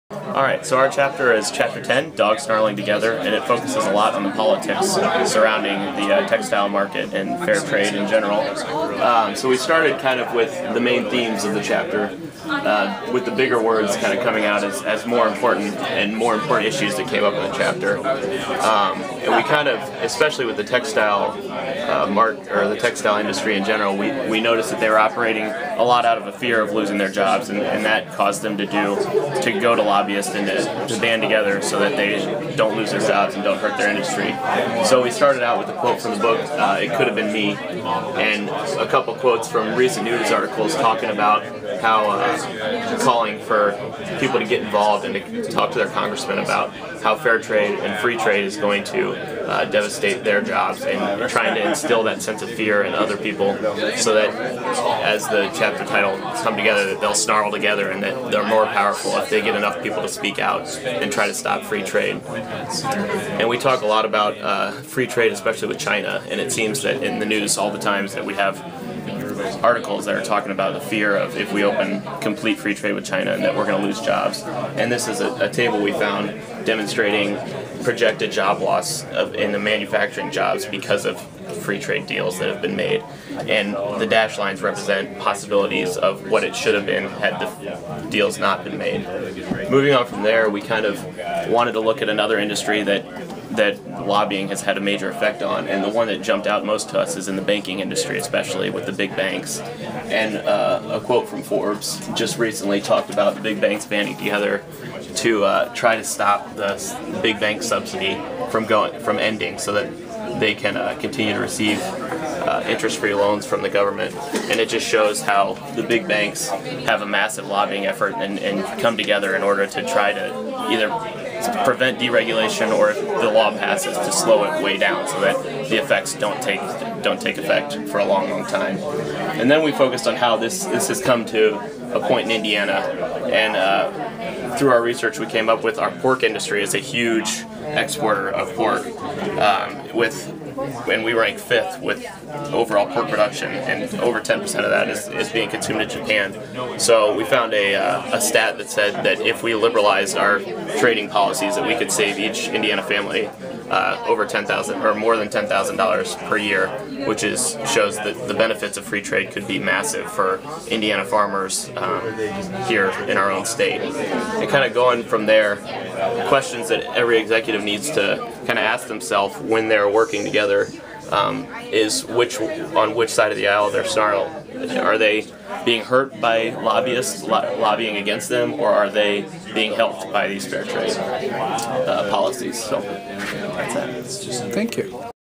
Chapter 10: Dogs Snarling Together
IU Kelley Business School, Indianapolis
D594 - Global Strategic Management
Professor Charles Dhanaraj
Travels of a T-Shirt in the Global Economy- Chapter 10: Dogs Snarling Together